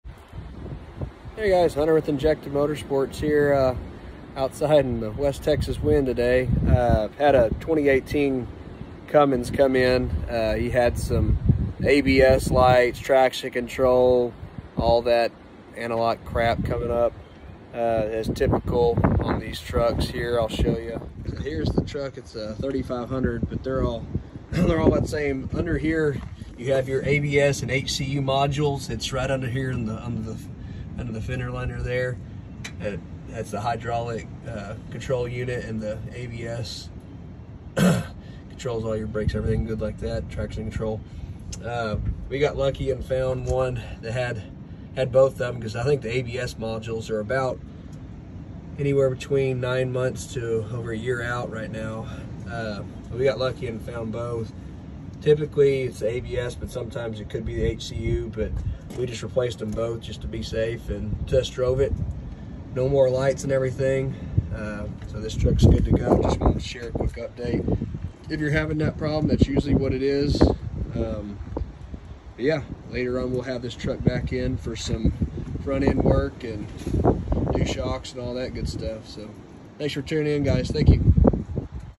2012-2018 Ram 2500/3500 ABS/HCU Module Repair Overview
Common issue on the new ram trucks is the Anti-Lock Brake System Hydraulic Control Unit. It causes the abs, traction control lights to come on and show faults in the system. We replaced both modules since it can be the HCU sometimes. After replacing, doing an auto bleed and driving the truck is fixed the issue. Please do a proper brake bleed procedure, if you do not know how then I would recommend taking it to a shop that can. Hope it helps, will post the part number here below from Mopar which is both parts combined! This did not require manufacturer tuning (it can in some cases).
Part# 68143491AA

Steps to remove & install:
Take driver side fender liner out,  then you'll remove the ABS cover and plug (pull the white tab back and pull outwards, sort of like an ecm plug). ** In rare cases you can pull this plug and blow it out to remove dust etc and plug it back in and the codes will disappear.
Remove torx bolts and the ABS will come out (this is where you can test resistance if you want to see if the motor was open or closed.
* If you want to take the entire ABS/HCU module out then do the steps above minus the torq bolts and using a line wrench take off the 6 hydraulic lines on top of the HCU module. The entire assembly will then come out so you can take the ABS apart from the HCU.

For install just repeat the steps in reverse, assemble the abs to the hcu, stick back in the truck, connect the 6 hydraulic lines, plug in the abs harness, put the cover and bolts back in, then ready for your brake bleed procedure.

Shop Address:
8470 Loop 570
Wall Texas 76957

Hours of Operation:
Monday-Friday 9:00am - 5:30pm CT
Saturday-Sunday - Closed

 abs module repair